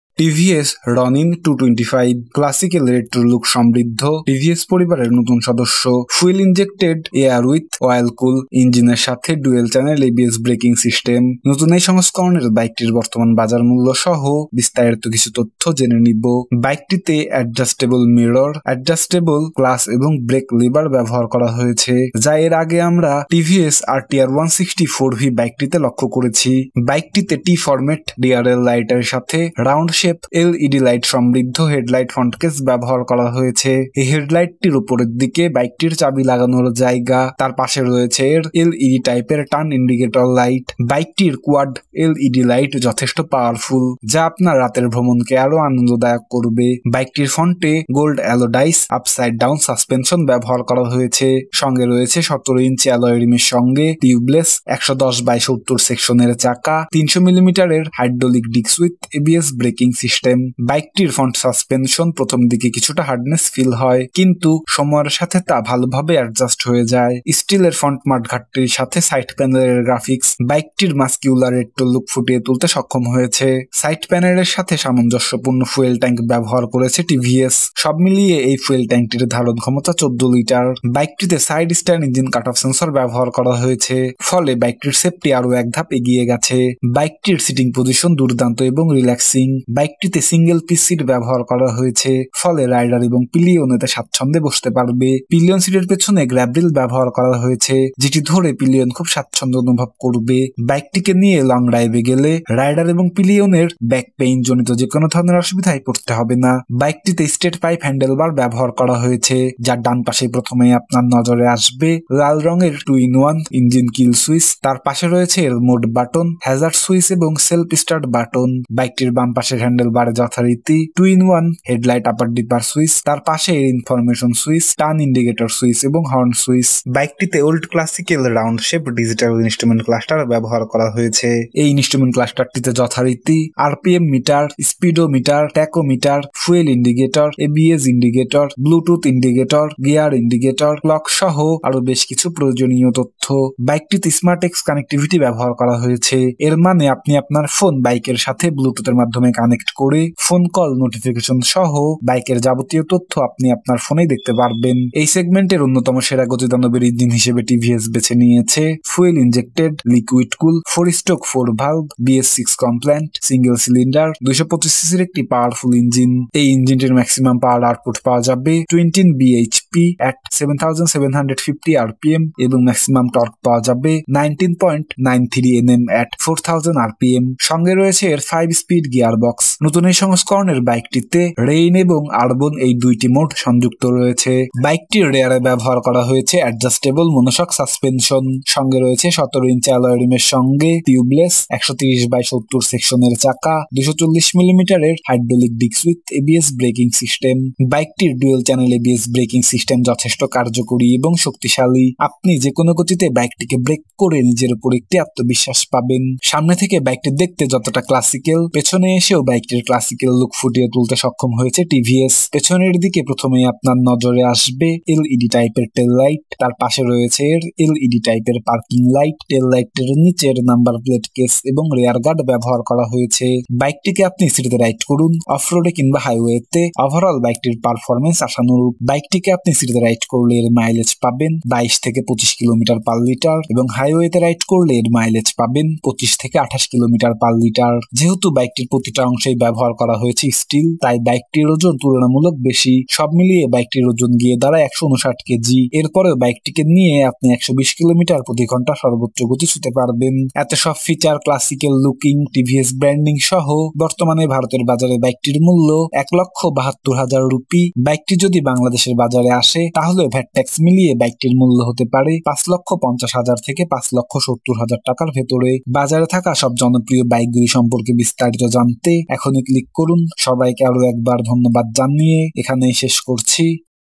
ক্লাসিক রেট্র লুক সমৃদ্ধ : [ TVS Ronin 225 ] : Review in Bangla...
TVS Ronin 225 is a new member of the TVS family with classic retro look, Fuel Injection, Air with Oil Cooled Engine Dual channel ABS Braking system with new version bike, let's know some details including the current market price of the bike.
The bike uses adjustable mirrors and adjustable class and brake levers, which we have seen earlier on the Tvs rtr 4v 160 special edition.
The quad LED lights of the bike are quite effective and powerful, which will make your night ride more enjoyable.
Due to the use of Wide Block Thread tire technology in the bike, you can enjoy special benefits in the field of off-road riding
Since steel is used in every part of the bike, its weight is relatively high, the total weight of the bike is 159 kg, still you can reach a maximum speed of 130 km per hour with the bike.
With all these features, muscular old classical retro look, TVS branding, the bike is currently priced at ₹ 1,49,000 in the Indian market.
If the bike ever comes to the market in Bangladesh then the price of the bike including VAT-tax may be between 5 lakh fifty to 5 lakh 70 thousand rupees.
  thanking everyone once again and ending here...

Quries Solved :

Specification: TVS Ronin 225

1. Details
CC Category 225 cc

2. Engine & Transmission
Displacement 126 cc
Engine Type Air & oil-cooled engine
Max Power 20 BHp @ 7750 rpm
Max Torque 19.93 Nm @ 4000 rpm
Gears  5
Clutch  Wet Multi -Plate
Engine Cooling Air & Oil Cooled
Fuel Supply Fuel Injection
Starting Method Electric Starter

3.Brakes, Wheels & Suspensions
Braking  ABS Brake System
Front Brake 300 mm Disc
Rear Brake 240mm Disc
Front Suspension Hydraulic Telescopic
Rear Suspension  Adjustable Monoshock

4.  Dimensions
5. Electricals
Headlight LED
Tail Light LED
Indicators Bulb

6. Top Speed and Mileage
Mileage (Average) ? (Approx)
Top Speed  130 KM/H (Approx)

7. Features
Engine Kill Switch Yes
Handle Type Pipe
Speedometer Digital
RPM Meter Digital
Odometer Digital
Seat type Split
Additional Features TFT Display, Bluetooth, SMART X CONNECT, intelliGO, Voice Assist, Engine inhibitor, Side-stand engine cut-off, 2 riding modes.